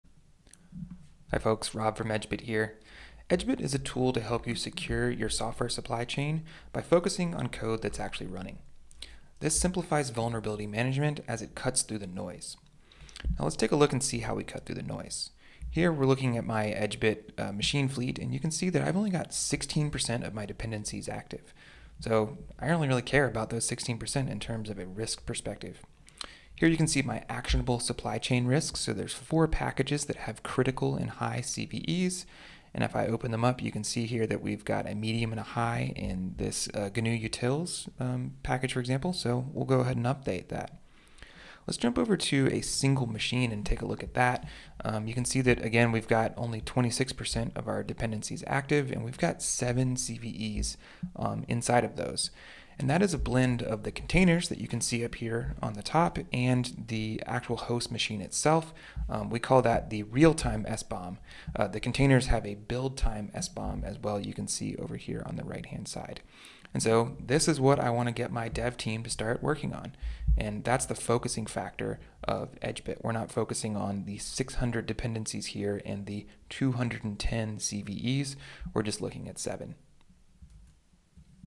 EdgeBit overview & the realtime SBOM
EdgeBit is a tool to secure your software supply chain that focuses on code that is actually running. This simplifies vulnerability management as it cuts through noise.